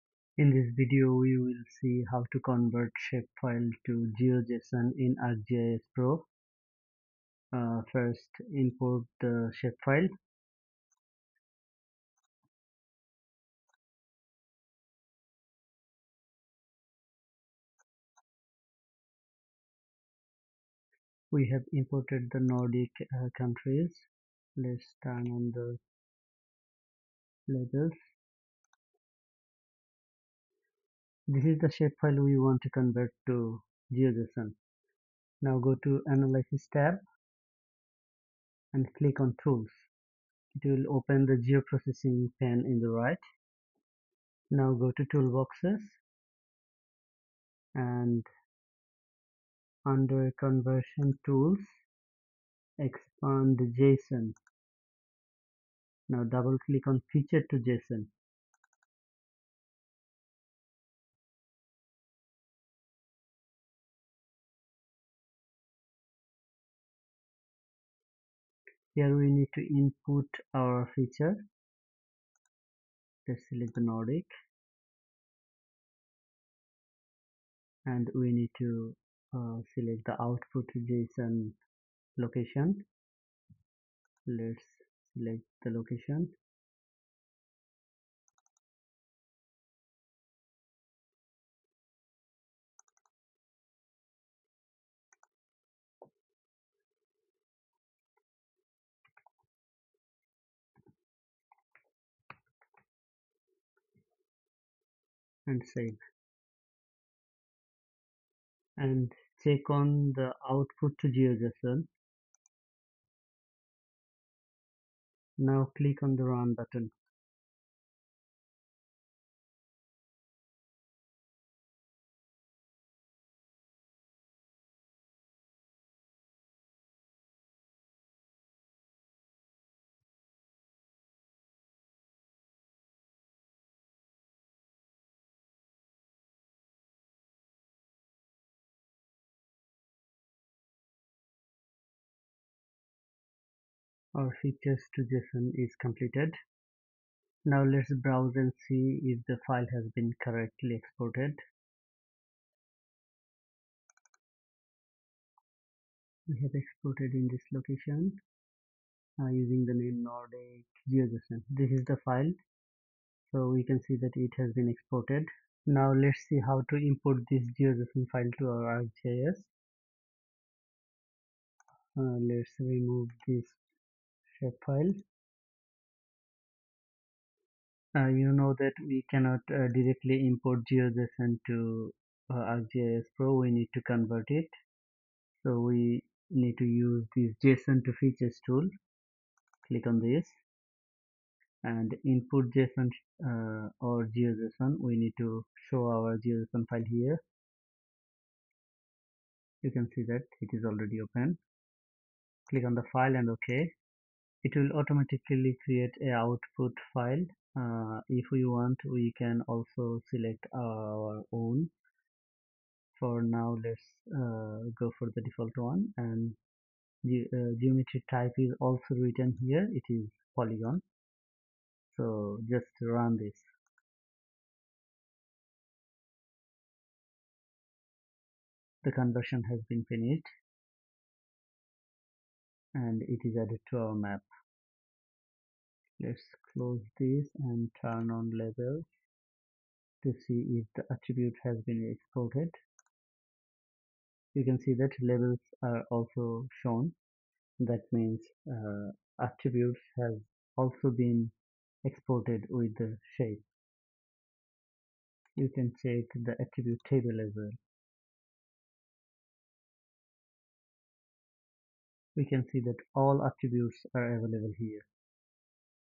Convert Shapefile to GeoJSON & Import in ArcGIS Pro | Beginner Tutorial
Learn how to convert a shapefile to GeoJSON and import GeoJSON into ArcGIS Pro with this step-by-step beginner tutorial! 🌍 GeoJSON is a widely used format for web mapping and GIS applications, making it essential for seamless data exchange.

In this video, you'll learn:
✅ How to convert a shapefile (.shp) to GeoJSON format
✅ Best tools and methods for accurate conversion
✅ How to import and visualize GeoJSON in ArcGIS Pro
✅ Tips for managing spatial data efficiently

Perfect for GIS beginners and professionals working with geospatial data formats!